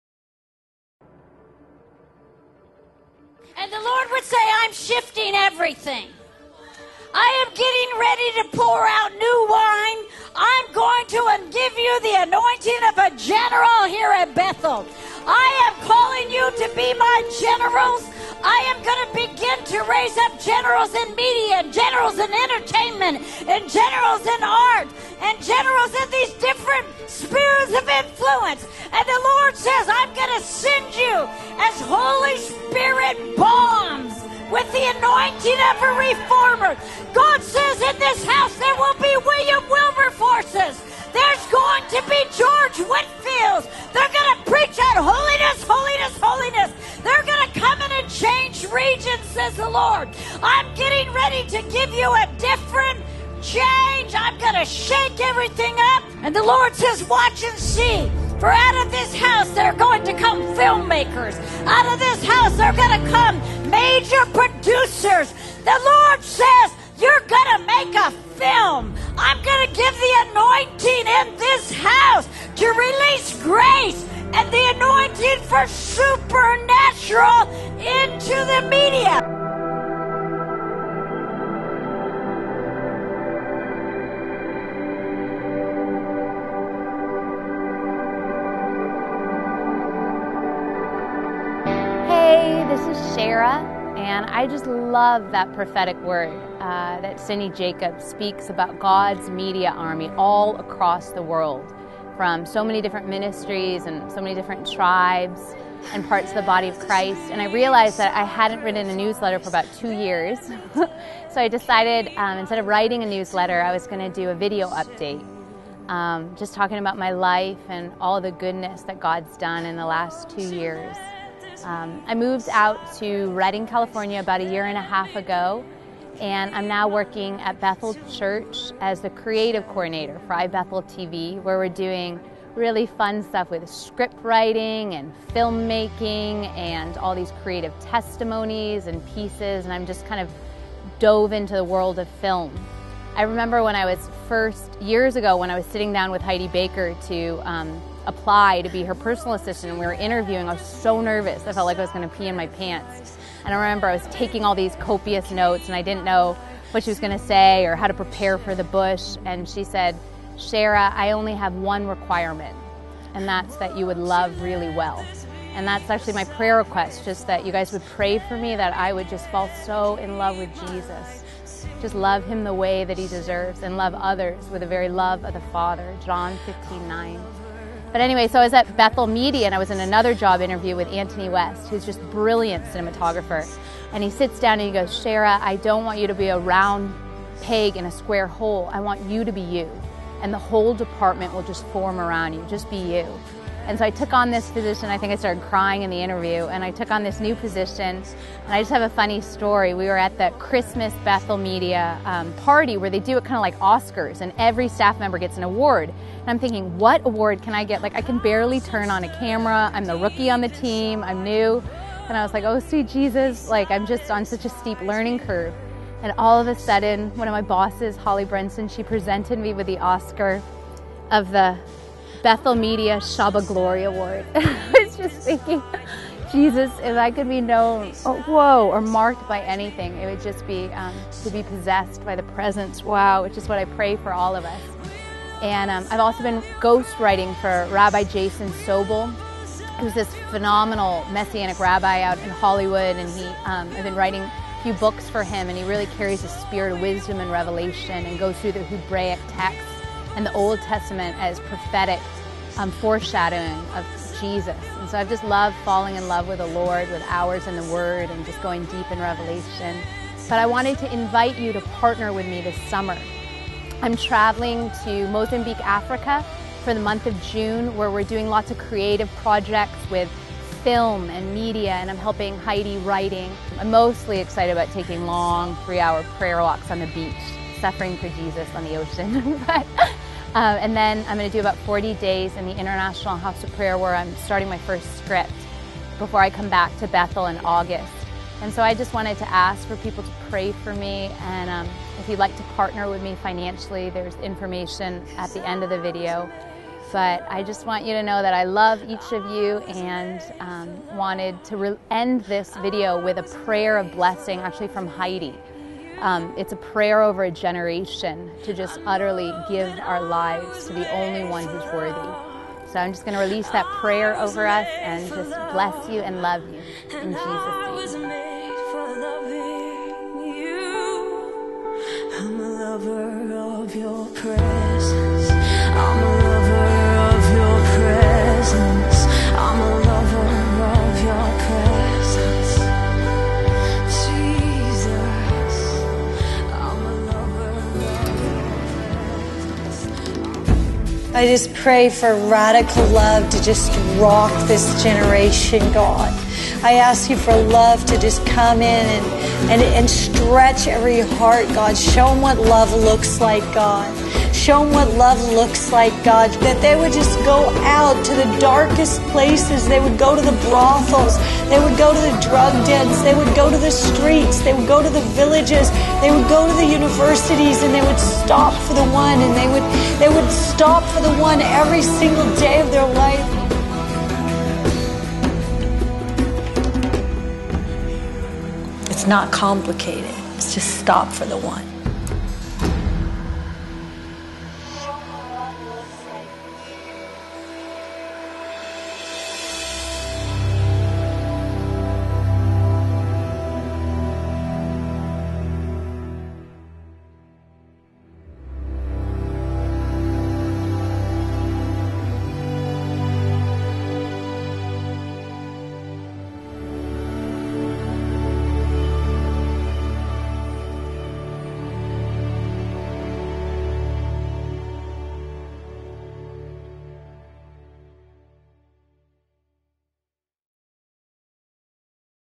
Shara Pradhan Update May 2012
Update from Shara Pradhan regarding her upcoming film and media blitz in Mozambique, Africa with Heidi Baker and at IHOP Kansas City.  After the summer blitz she will be returning to her post as creative coordinator for Bethel Media in Redding, CA.